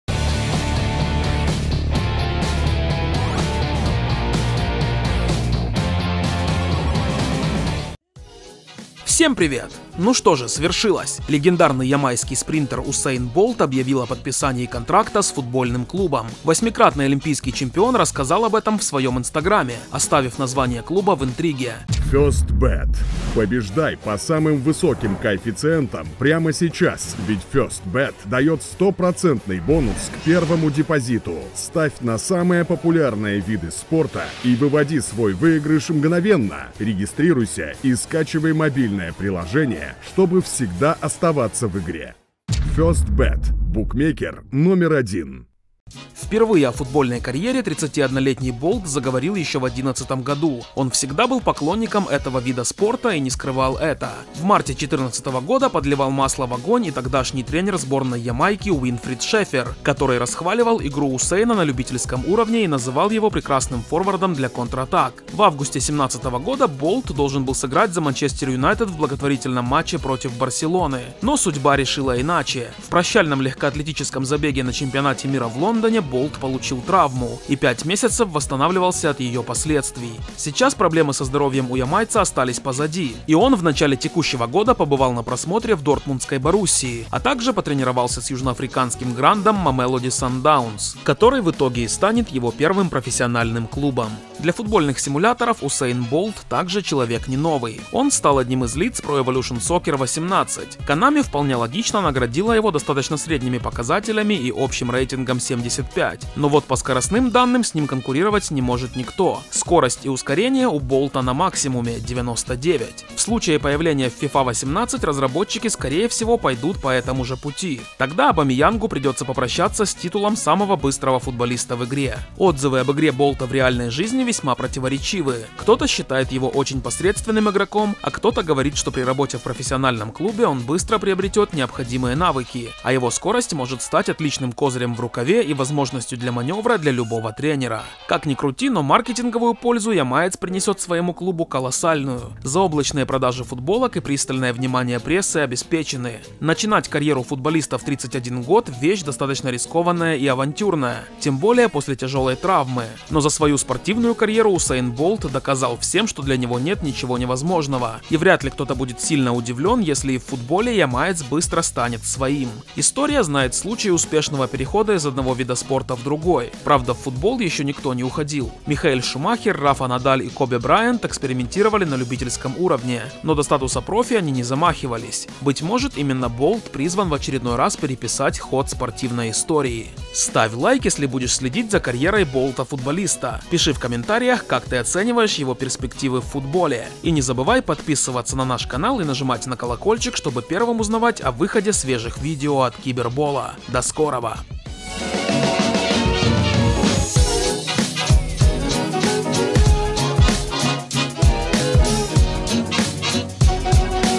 УСЭЙН БОЛТ – ПРОФЕССИОНАЛЬНЫЙ ФУТБОЛИСТ l ЖДЕМ В FIFA 18!
Всем привет!
Ну что же, свершилось. Легендарный ямайский спринтер Усэйн Болт объявил о подписании контракта с футбольным клубом.
8-кратный олимпийский чемпион рассказал об этом в своем инстаграме, оставив название клуба в интриге.
Впервые о футбольной карьере 31-летний Болт заговорил еще в 2011 году. Он всегда был поклонником этого вида спорта и не скрывал это.

фифа 18, фифа 18 Болт, Усэйн Болт.
fifa 18, fifa 18 bolt. Usain Bolt